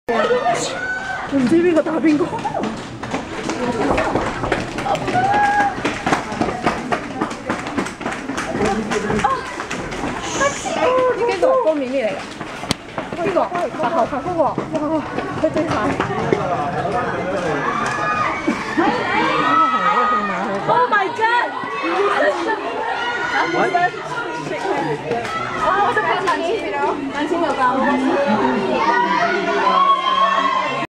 BEAST, 4 Minute & G.NA - departure from KLIA @ 28 June 2013
leaving Kuala Lumpur on 28 June 2013. They were in Kuala Lumpur for the AIA-Kpop concert.